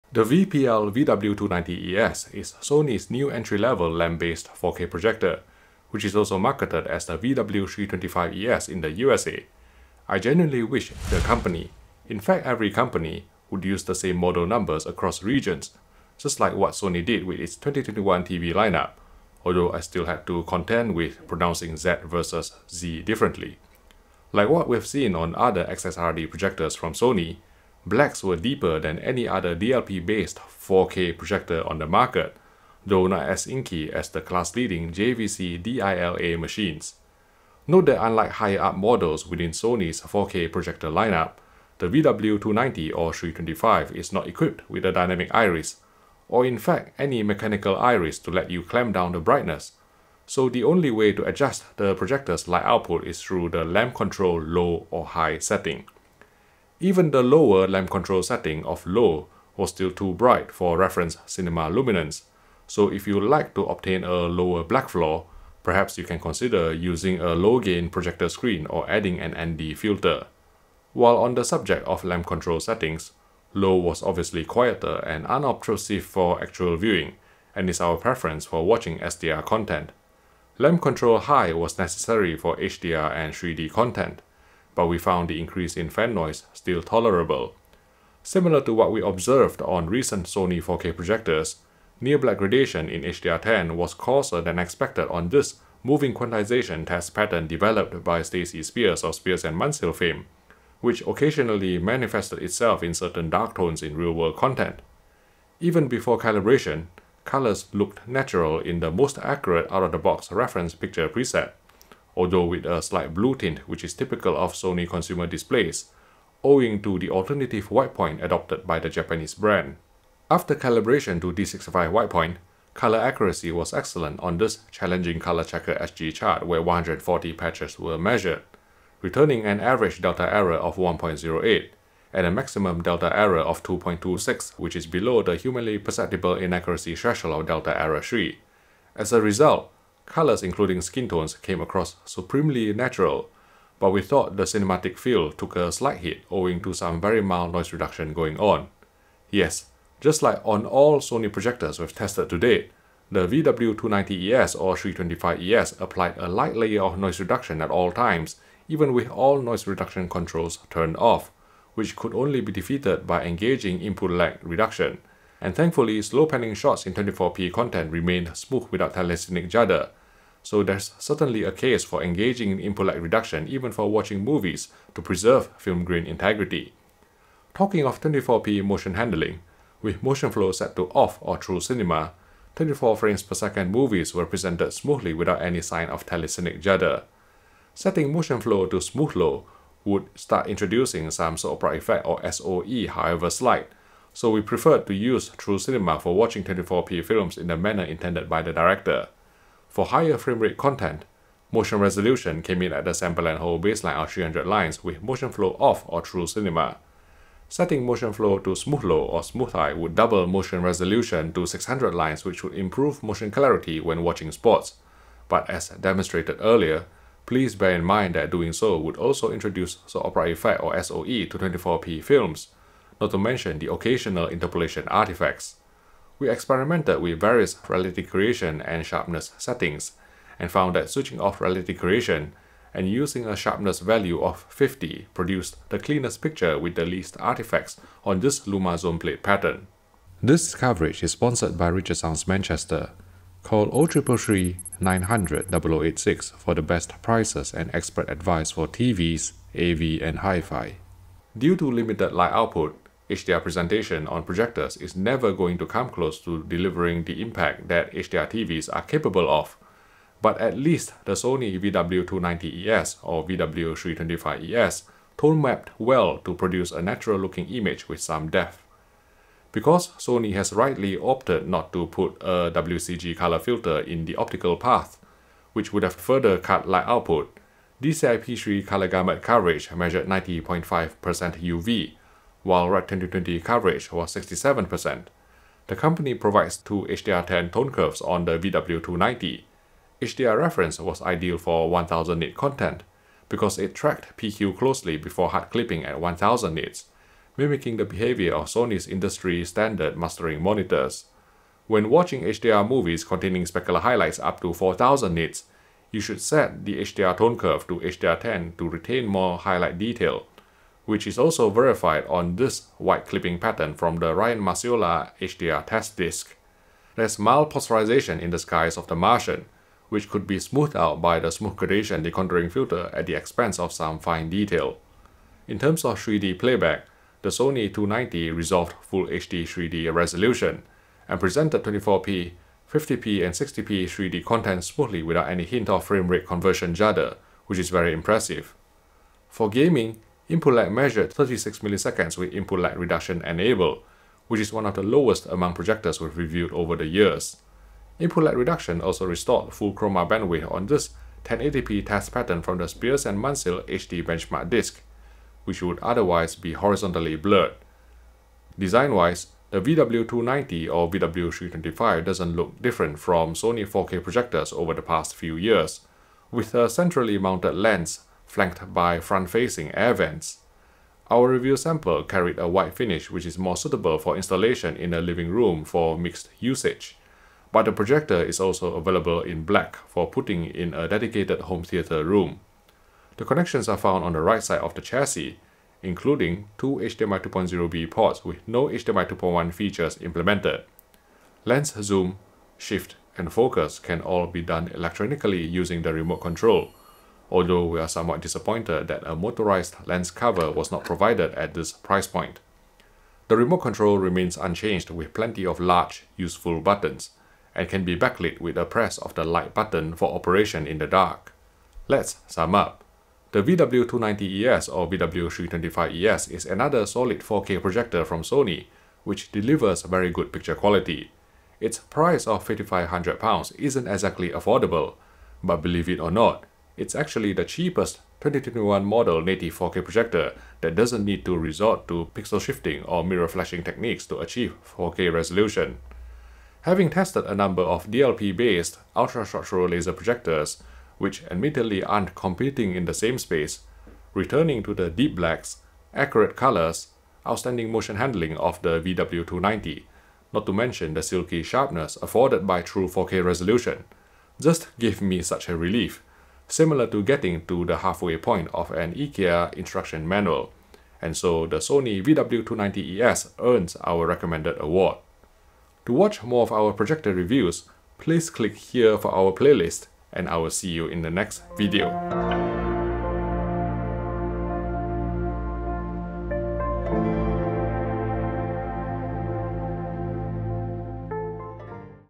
Sony VPL-VW290ES (VPL-VW325ES) 4K Projector Review
We review the Sony VPL-VW290ES 4K projector, which is also marketed as the VPL-VW325ES in the USA. With a launch price of £5,500, the VW290 is Sony's most affordable native 4K projector for 2021.

Topics covered in this Sony VW290ES/ VW325 review include contrast performance, colour accuracy, motion handling, video processing, HDR, as well as input lag for gaming.